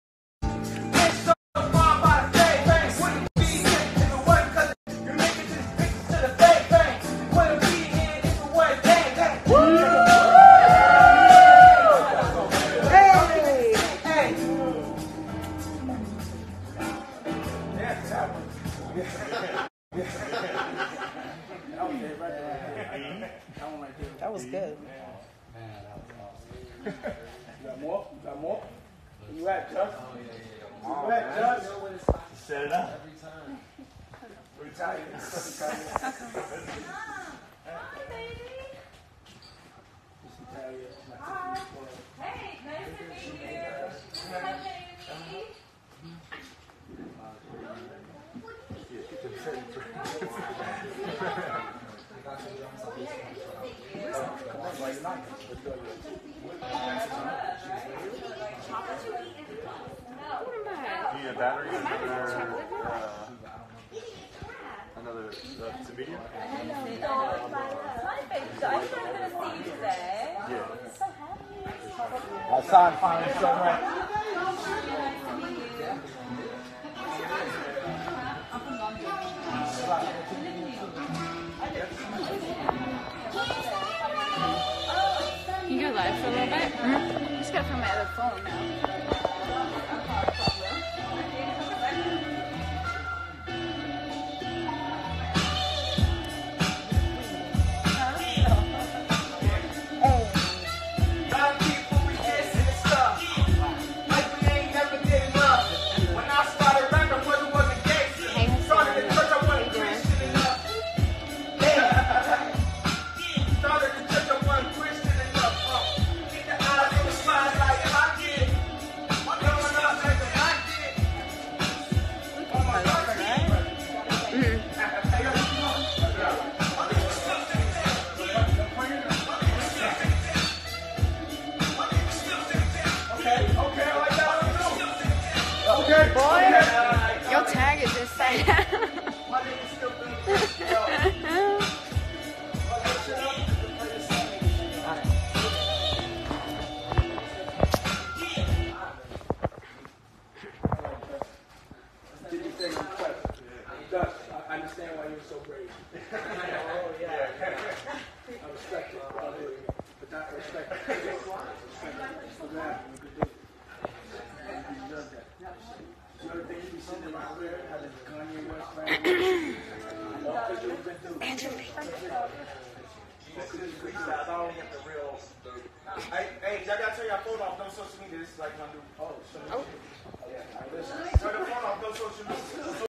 KanyeWest In The Studio With damedash Dame Dash
He got him out his shell